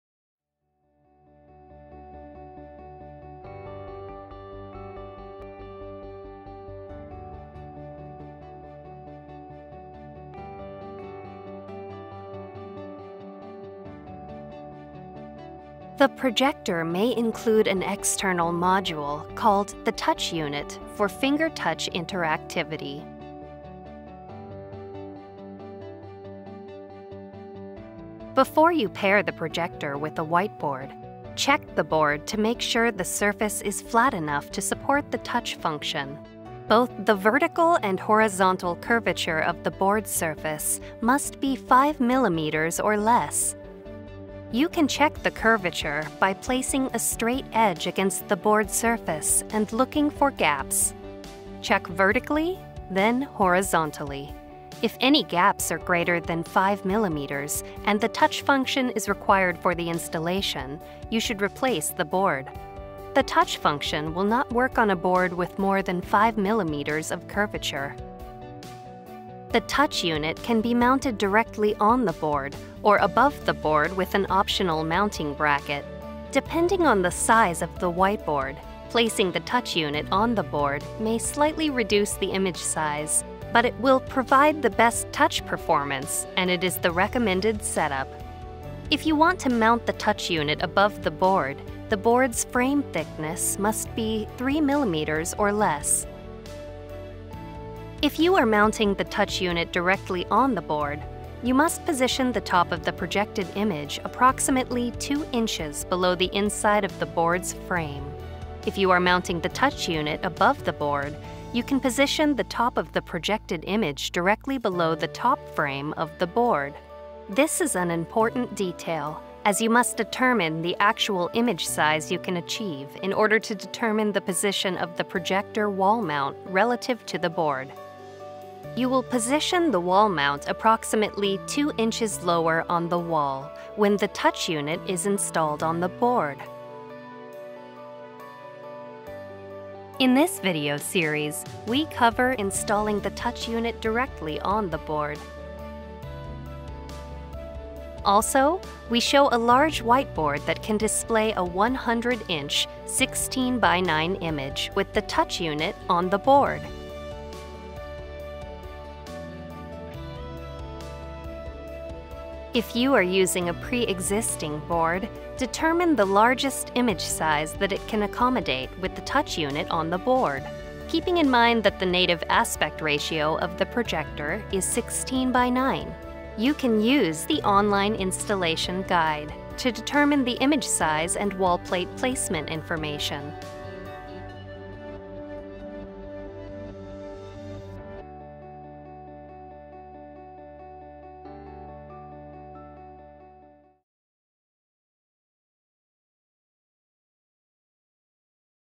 EB-1485Fi Step 7 - Finger touch unit installation requirements
Step 7 of 11 in the series on how to install an EB-1485Fi. Watch this video to find out how to install the Finger Touch Unit for the EB-1485Fi interactive display solution. The information contained in this video is also applicable to the EB-725Wi and EB-735Fi.